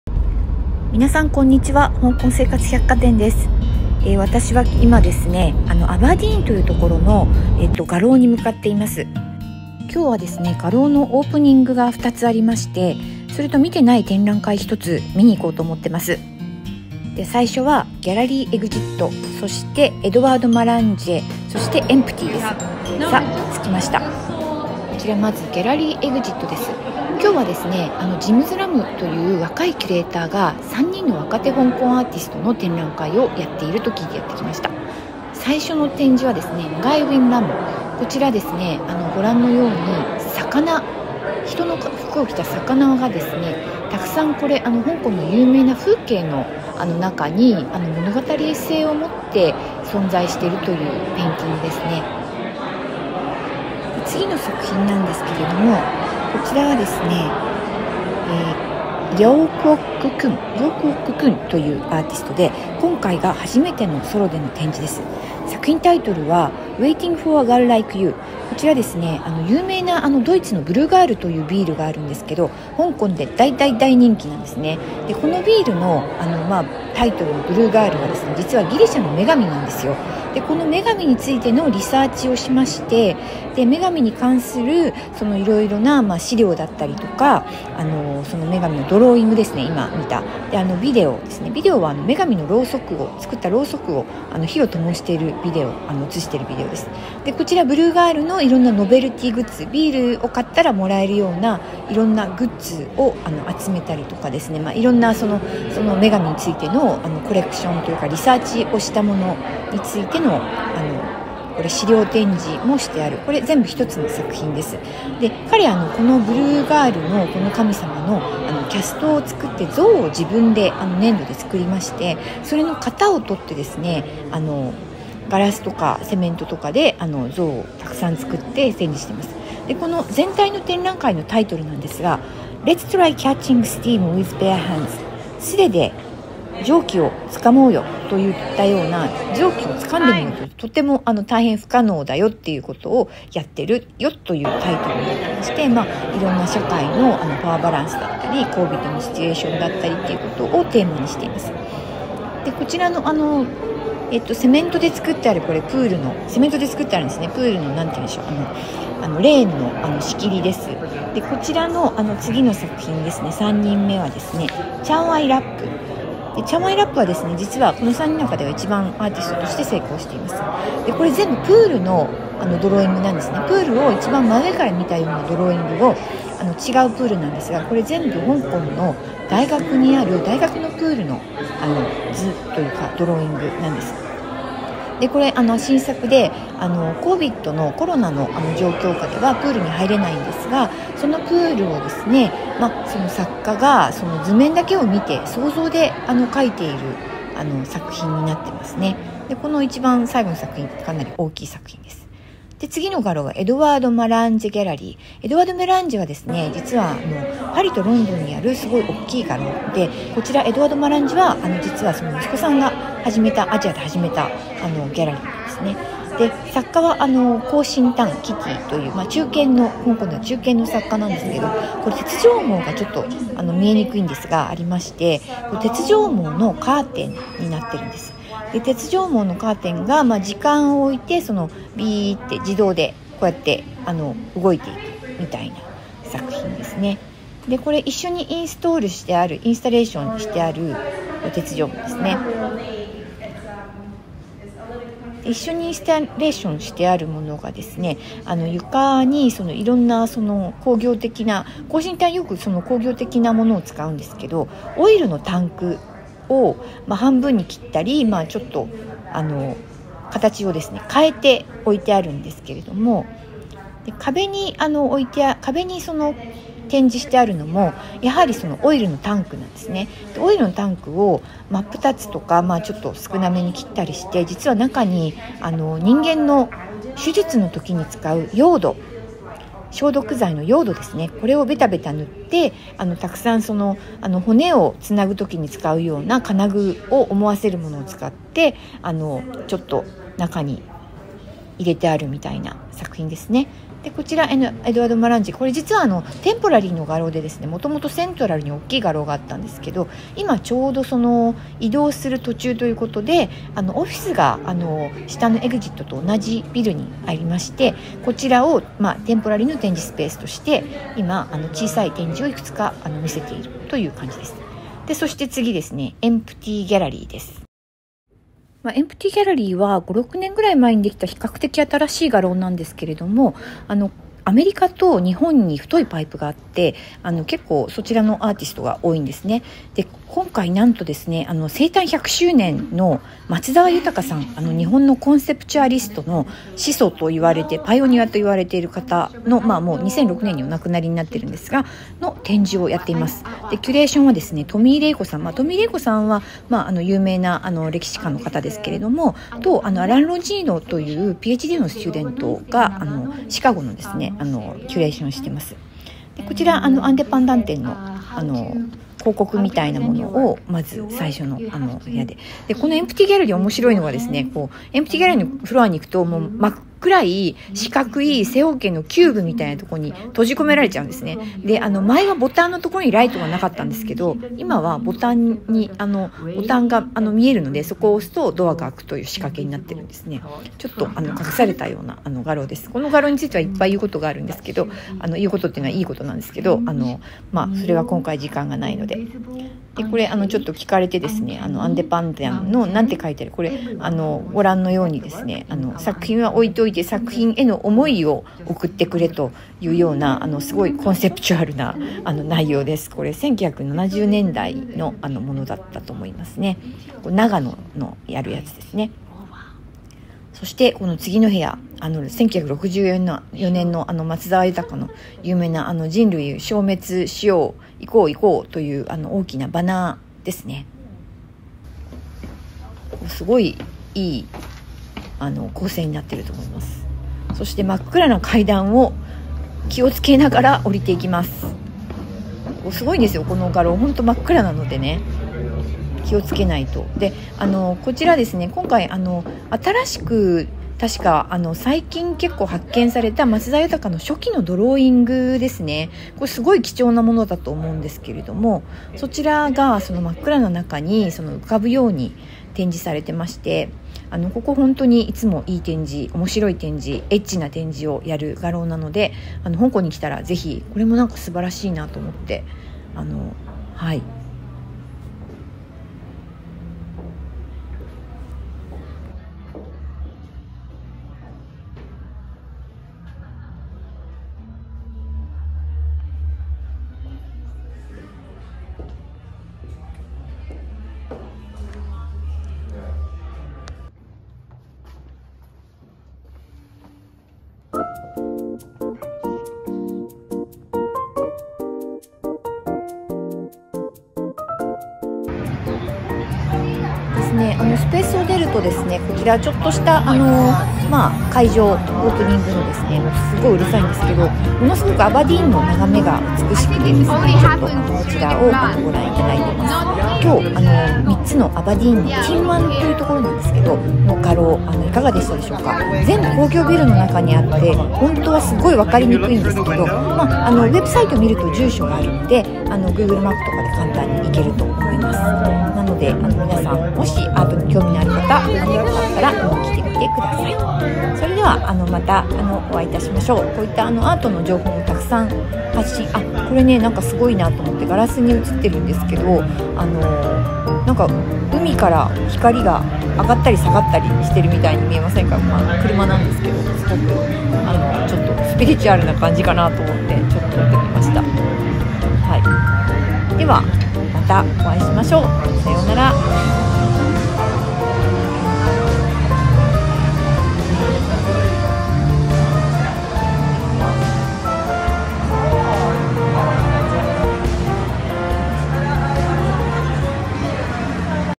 Empty とExit、工業ビルのギャラリーオープニング！松澤宥、香港若手アーティスト Yutaka Matsuzawa and young artists in Hong Kong!
香港、アバディーンの工業ビル街にある画廊のオープニングです。若手アーティストから生誕１００周年の松澤宥の展覧会、３つのギャラリーをご紹介します。

香港島の南側、アバディーンにはたくさん画廊がありますが、こちらの３軒は少しはなれた田湾というエリアにあります。